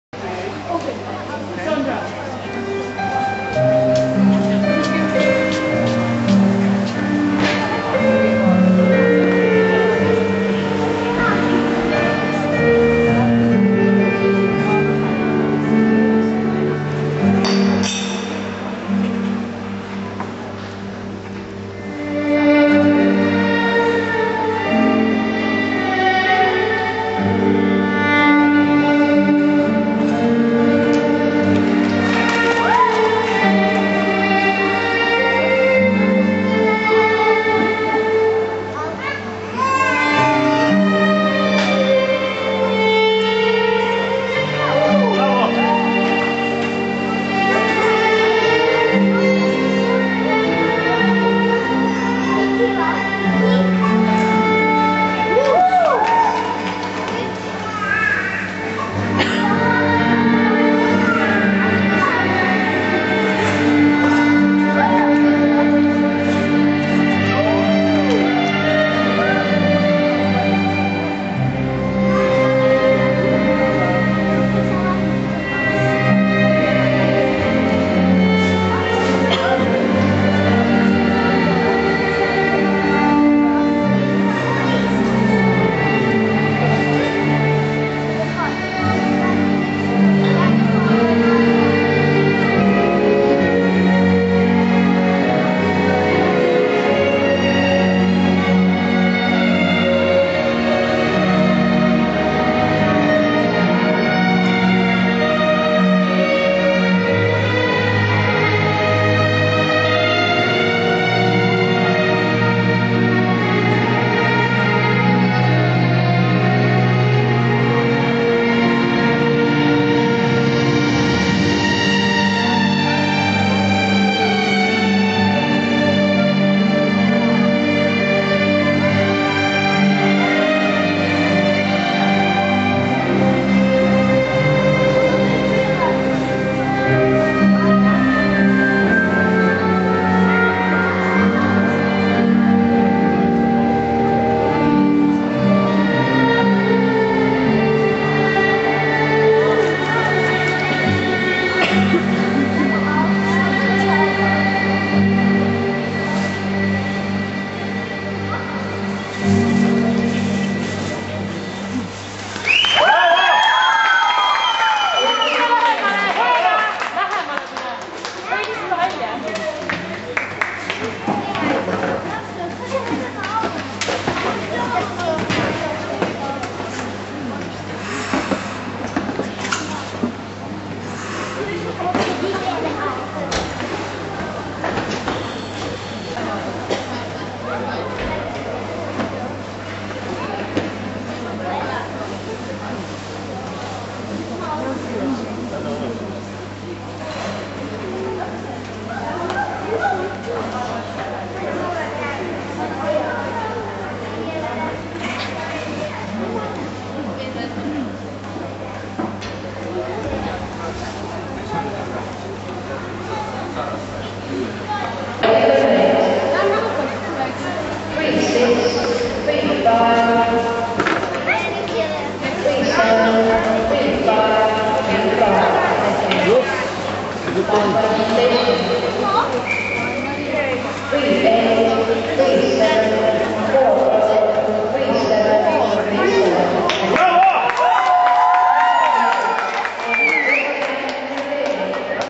2006_12_01, Duygu Sen; Tallinn Trophy Springs Girls B
Duygu Sen from Turkey, standing 3rd place